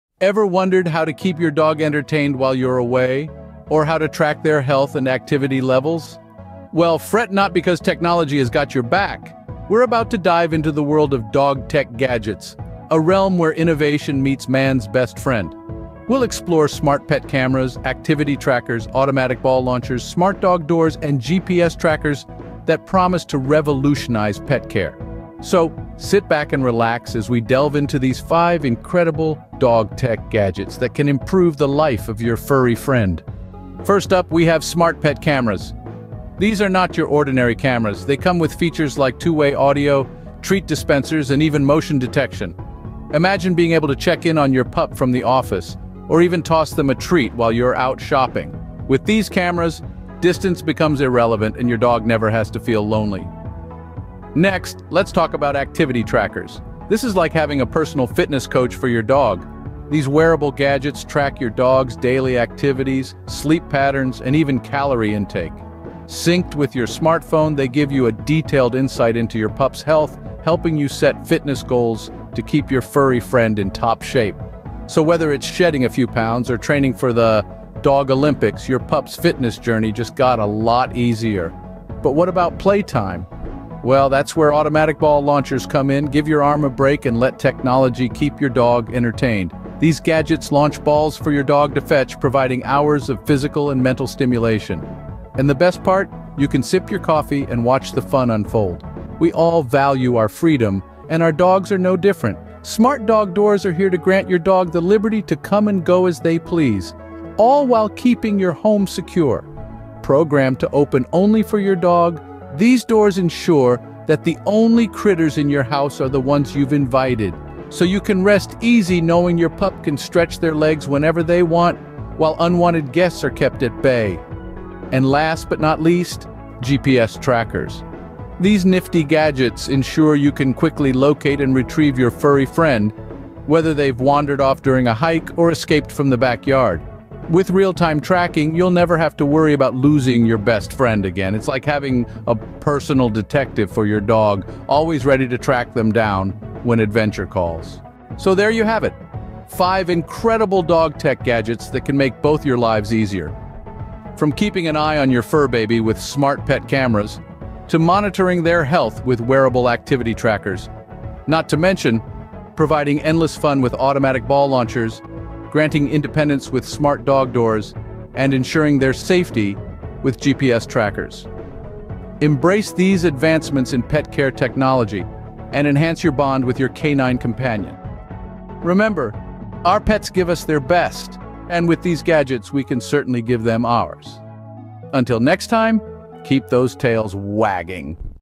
Top 5 Gadgets for Your Furry Friend
🐾 Calling all tech-savvy dog lovers! 🐾

Get ready to bark up the right tree with the most paw-some gadgets for your furry friend! From gadgets that'll make tails wag to inventions that'll have your pup begging for more, we're diving deep into the world of canine innovation in this woof-tastic video!

Join us as we sniff out the latest and greatest gadgets designed to delight both you and your dog. From high-tech toys that'll keep your pup entertained for hours to genius grooming gadgets that'll have your pooch looking like they just stepped out of a doggy spa, we've got it all covered!

Watch in awe as we showcase gadgets that are guaranteed to make your life easier and your dog's life even more fabulous. Trust us, once you see these gadgets in action, you'll wonder how you ever lived without them!

So grab your pup, settle in, and prepare to be amazed by the world of dog gadgets like never before. 🎥🐶